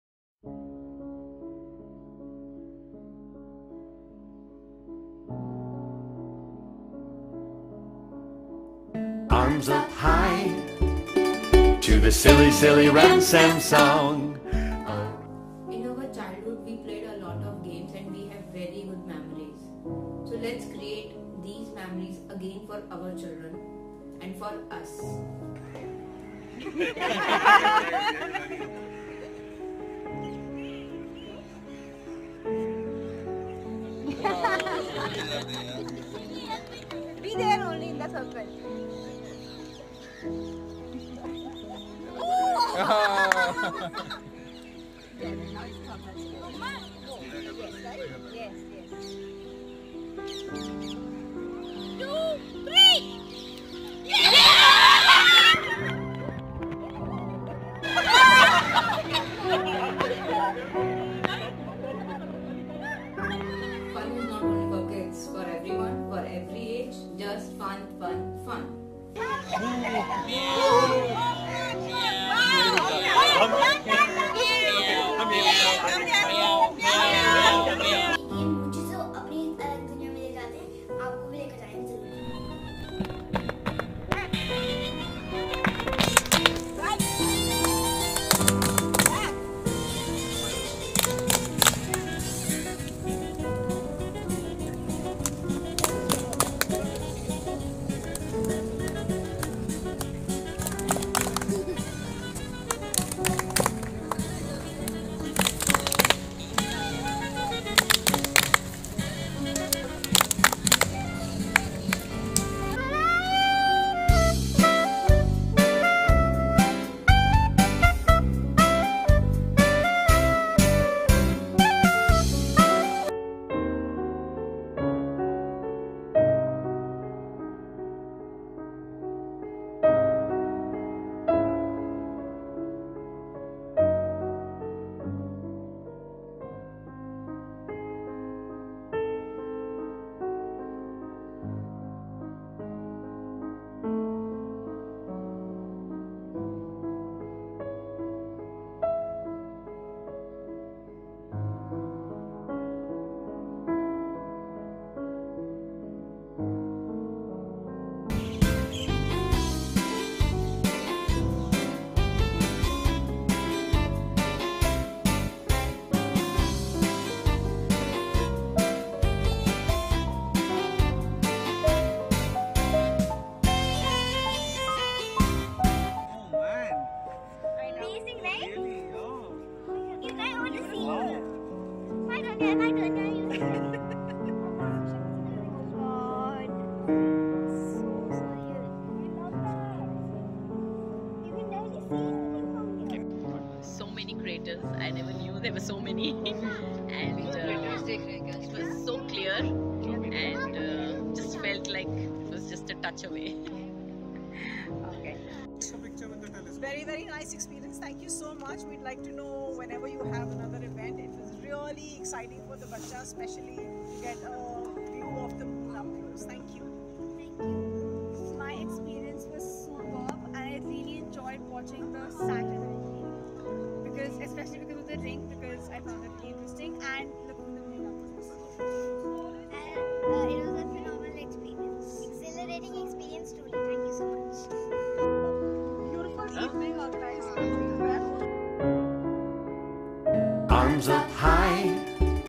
Fun & Wonder evenings
बचपन की दुनिया में
Fun Games & Circle Dances:
Let`s spend an evening playing interesting non competitive, non judgemental games, & having real fun without fear of loosing and being left out!

Food Break in between for 15-30 minutes, as convenient to all:
WE will have a Food break in between Fun & Wonder sessions.
You can bring your own Food or explore more food /snack options.

Wonder Astronomy - Journey into the Night SKY:
NIGHT SKY Observation with help of High Powered Professional Telescope
Telescopic observation of Wonders of the Universe, in the Night SKY.
This evening, We will take you all to a Journey to two of our most remarkable planets - Jupiter & Saturn. We get a chance to look up closer. Jupiter & her 4 Biggest Moons. Saturn with her Rings...

Our other evenings are - Journey to the Moon.

Our all Explorations are going to be once in a Lifetime Observations & Explorations! Our evenings are for everyone... for Child in all of us!! So, come along with your full family - your child